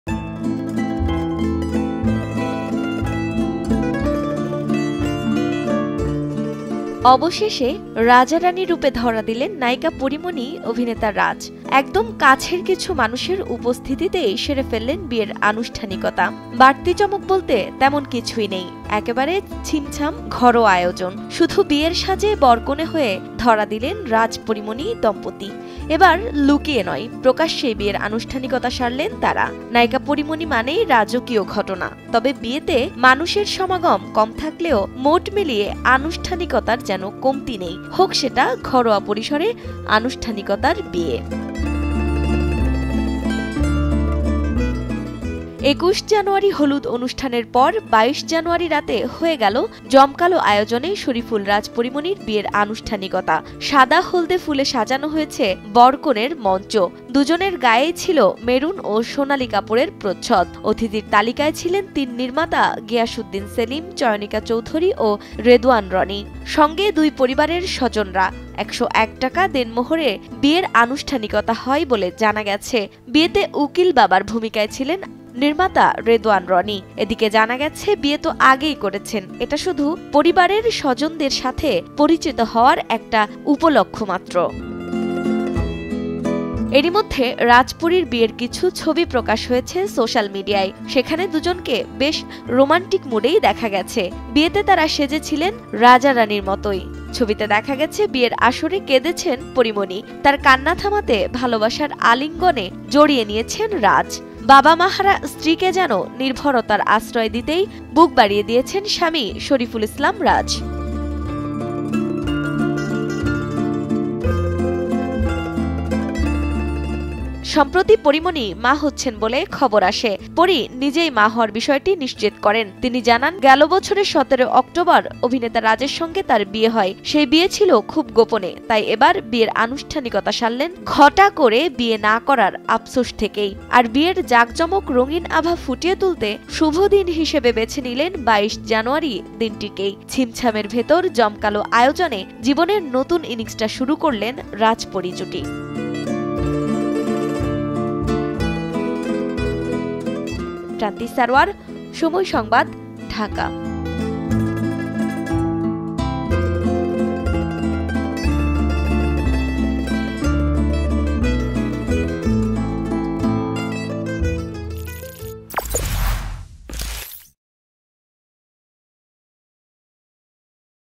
বিয়ের উকিল বাবা রেদওয়ান রনি | Pori Moni | Shariful Raj | Somoy Entertainment
অবশেষে রাজা-রানী রুপে ধরা দিলেন নায়িকা পরীমনি অভিনেতা রাজ। একদম কাছের কিছু মানুষের উপস্থিতিতেই সেরে ফেললেন বিয়ের আনুষ্ঠানিকতা। বাড়তি চমক বলতে তেমন কিছুই নেই, একেবারে  ছিমছাম আয়োজন। শুধু বিয়ের সাজে বর-কনে হয়ে ধরা দিলেন রাজ-পরীমনি দম্পতি। এবার লুকিয়ে নয় প্রকাশ্যেই বিয়ের আনুষ্ঠানিকতা সারলেন তারা। নায়িকা পরীমণি মানেই রাজকীয় ঘটনা, তবে বিয়েতে মানুষের সমাগম কম থাকলেও মোট মিলিয়ে আনুষ্ঠানিকতার যেন কমতি নেই, হোক সেটা ঘরোয়া পরিসরে আনুষ্ঠানিকতার বিয়ে